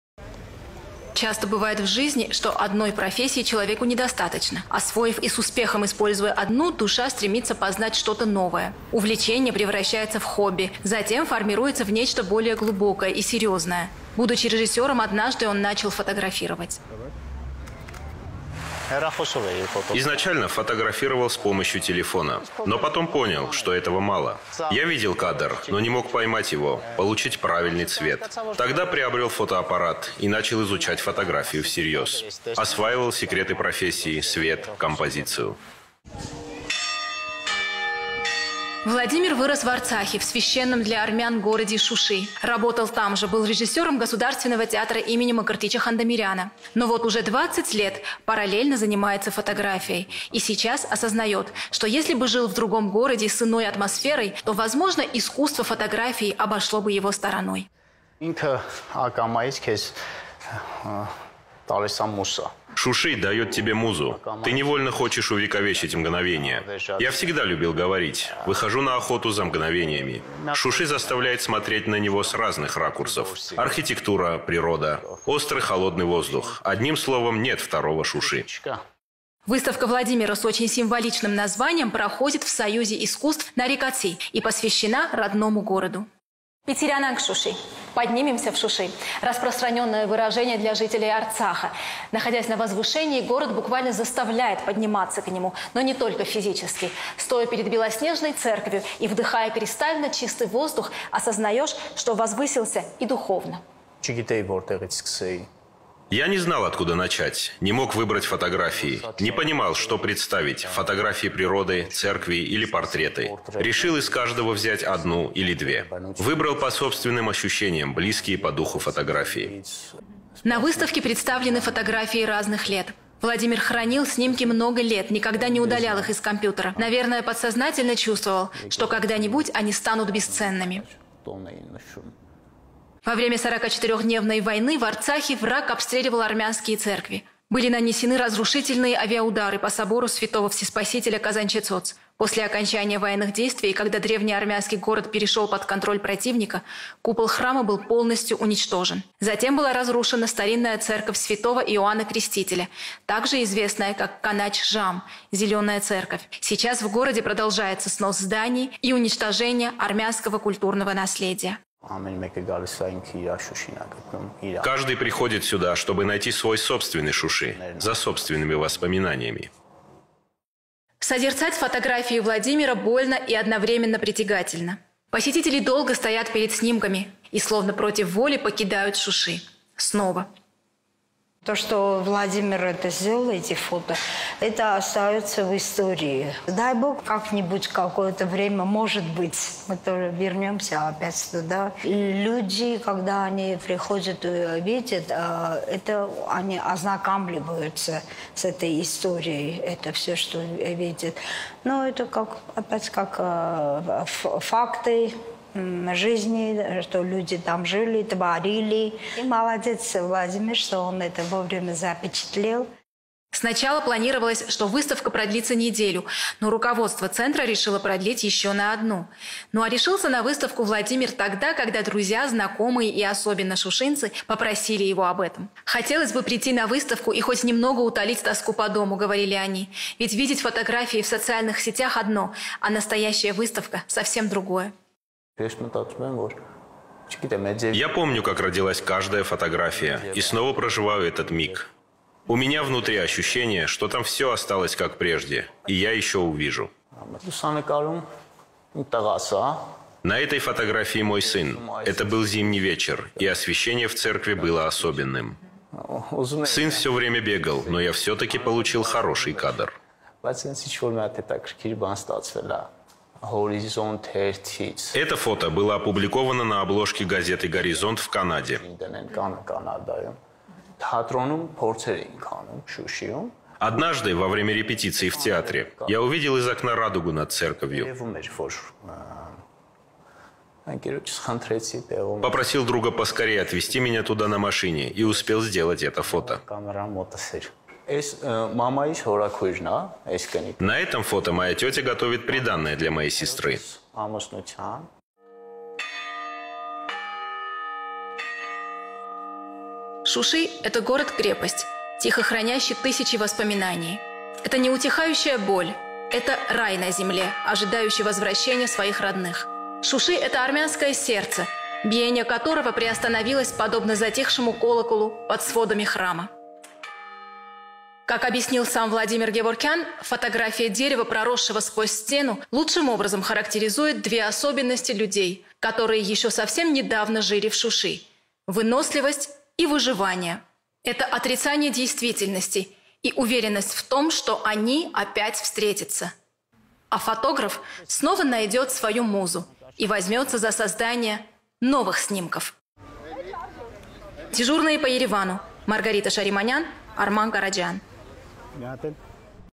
Поднимемся в ШУШИ - Азербайджанцы стирают армянство
Азербайджанцы пытаются стереть историю древнего армянского города Шуши, они нагло разрушали армянские церкви, тут мы многое понимаем из программы, созданной для российского канала.